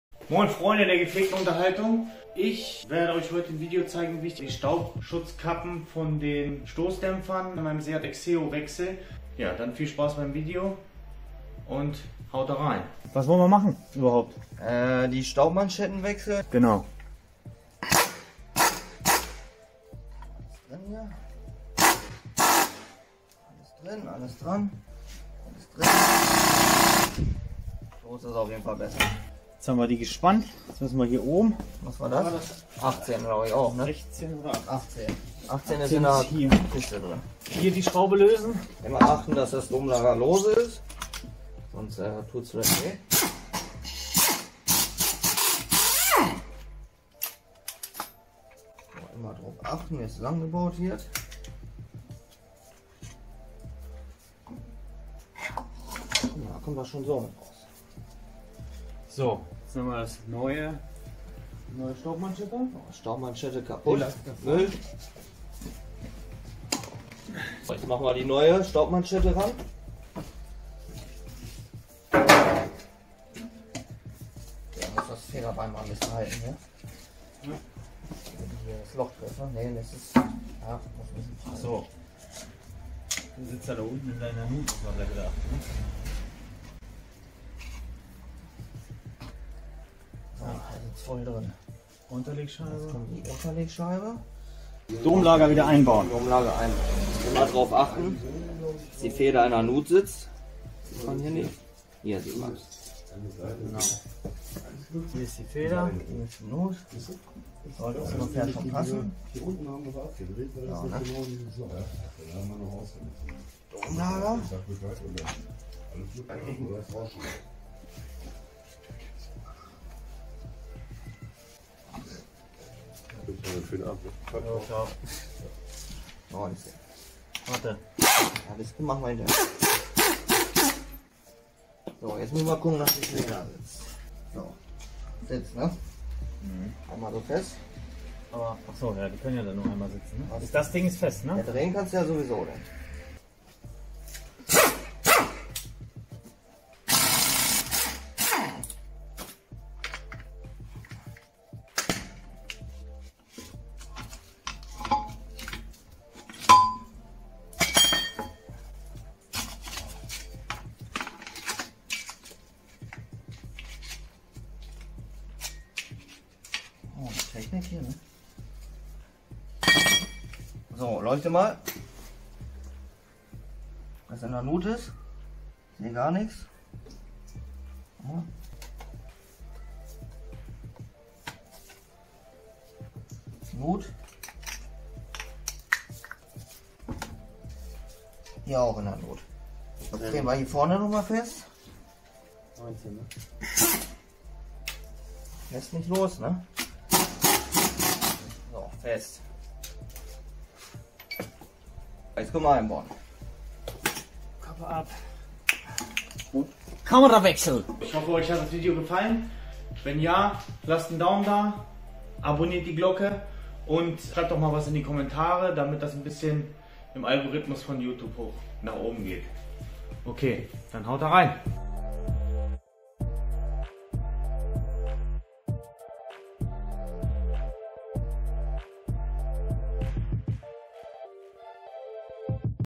Staubschutz-Kappe Stoßdämpfer wechseln vorne Seat Exeo 2.0L
heute zeige ich euch wie man die Querlenker am Seat exeo wechselt.

die Anzugsdrehmomente bitte dem vag Katalog entnehmen.

dies ist kein Lehrvideo ich mache diese Tätigkeiten mit bestem Wissen und Gewissen.

schaut euch auch gerne unsere anderen Videos auf dem Kanal an

liken teilen und anbieten nicht vergessen

wirselbermacher